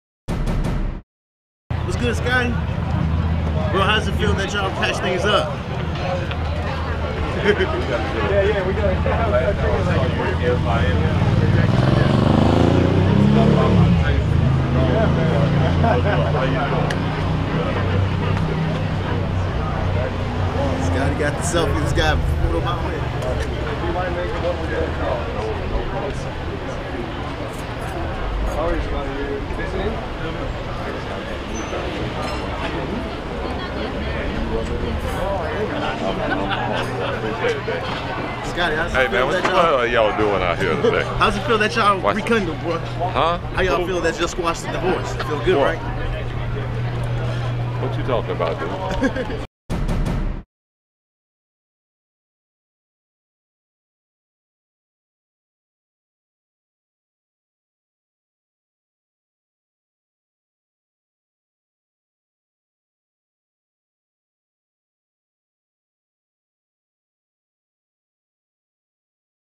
Scottie Pippen Fanboys Out After Spotting 'Tennis GOAT' Novak Djokovic
Scottie Pippen LOVES Novak Djokovic -- and wasn't afraid to show it yesterday at lunch.